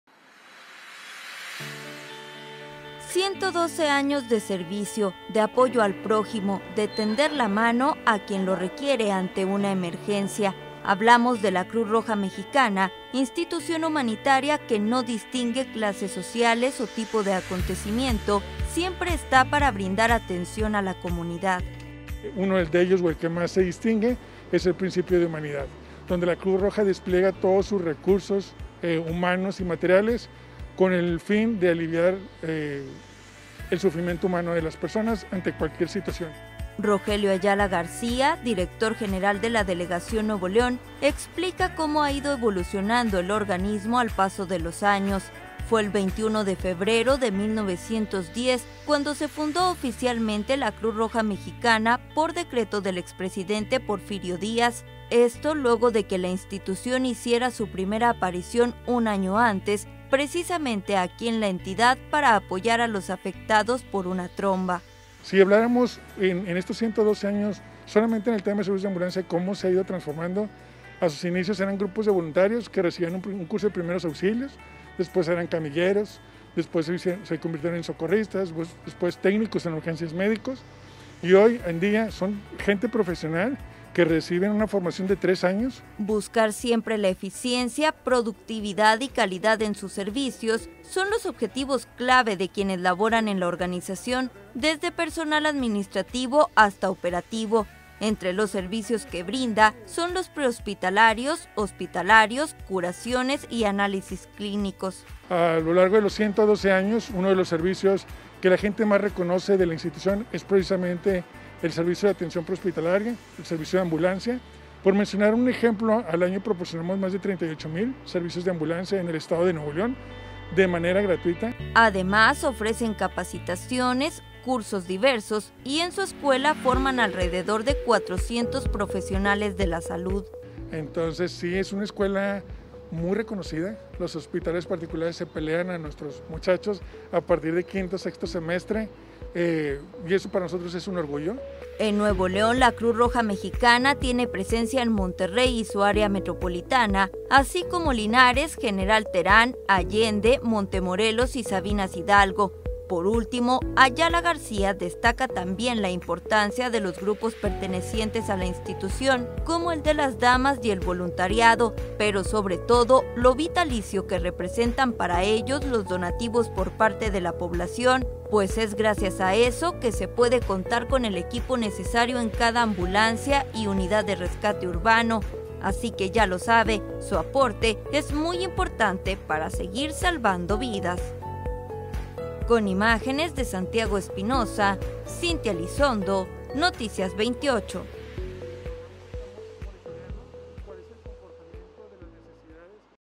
Cruz Roja Mexicana, 112 años de ayuda
Este 21 de febrero la Cruz Roja Mexicana cumplió 112 años de brindar ayuda a la población. Su labor invaluable y sus valores de disciplina y humanidad han fortalecido a esta institución humanitaria a través de los años.